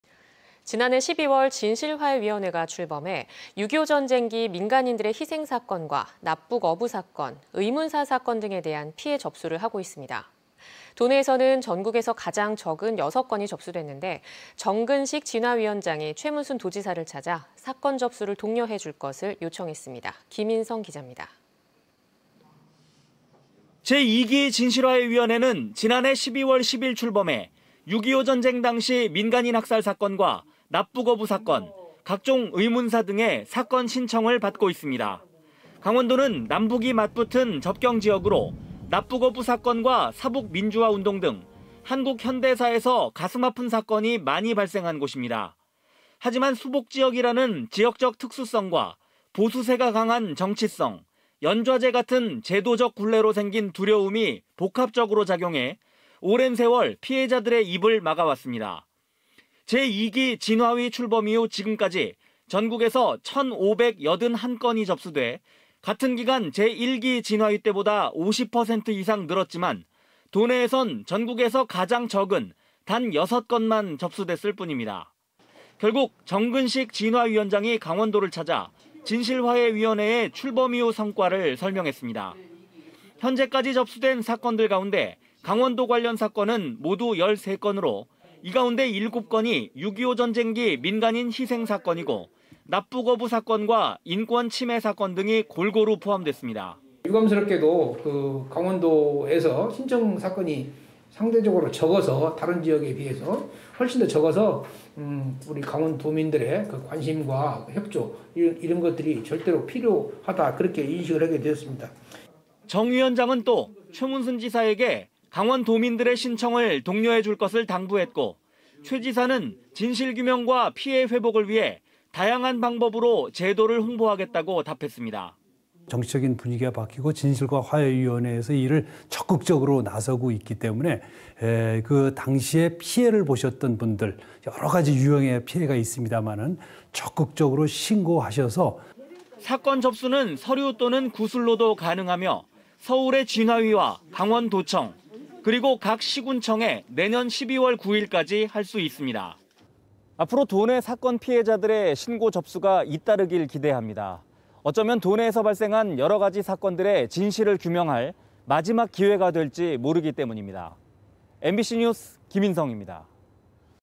[뉴스리포트]강원도 과거사 진상규명 신청 저조/210203
◀ＡＮＣ▶
 지난해 12월 진실화해위원회가 출범해
6.25 전쟁기 민간인들의 희생 사건과 납북 어부 사건, 의문사 사건 등에 대한 피해 접수를

 도내에서는 전국에서 가장 적은 6건이
접수됐는데,
정근식 진화위원장이 최문순 도지사를 찾아
사건 접수를 독려해줄 것을 요청했습니다.